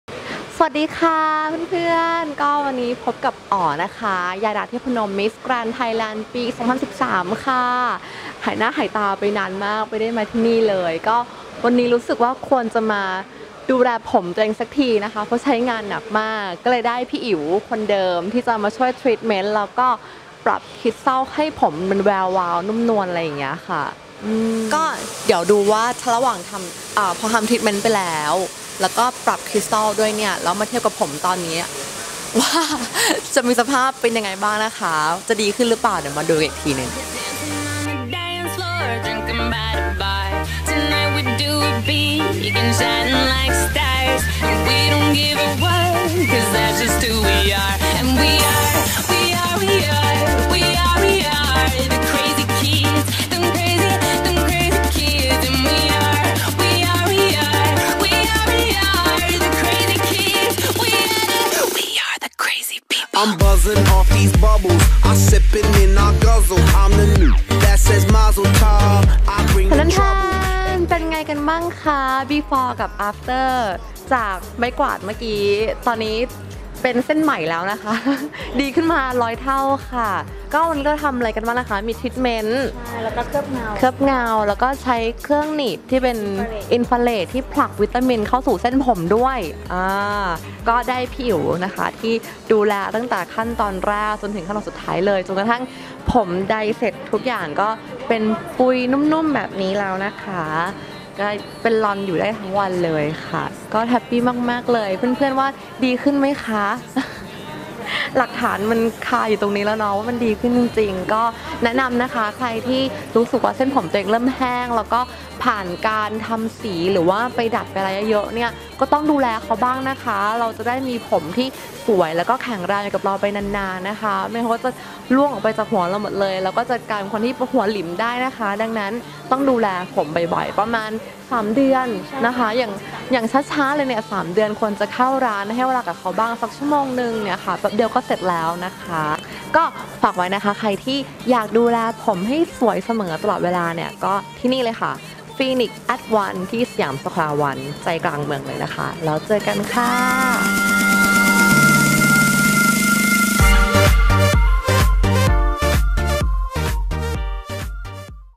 Phoenix at One คุณอ๋อ มาเคลือบเงา พร้อมทำทรีทเม้นท์ โดยช่างอิ๋ว ค่าา
คุณอ๋อ MissGrandThailand2013 มาเคลือบเงา พร้อมทำทรีทเม้นท์ ให้ผมนุ่มลื่น สุขภาพดี โดยช่างอิ๋ว จะออกมาสวยแค่ไหน ไปดูกันเลยค่าาา